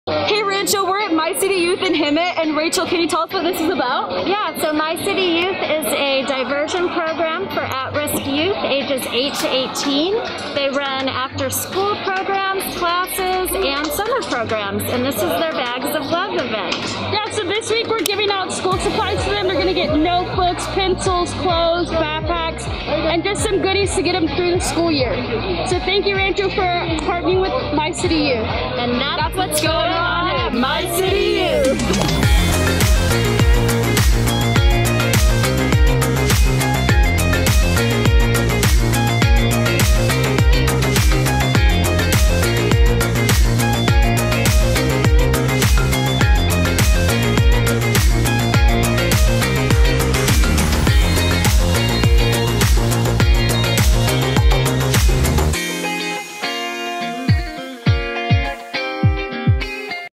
Bags of Love at My City Youth
On July 16 Rancho Murrieta and Temecula joined forces with others and delivered "bags of love" at My City Youth in Hemet, CA. Thanks to everyone who donated and served to advance the cause of Christ!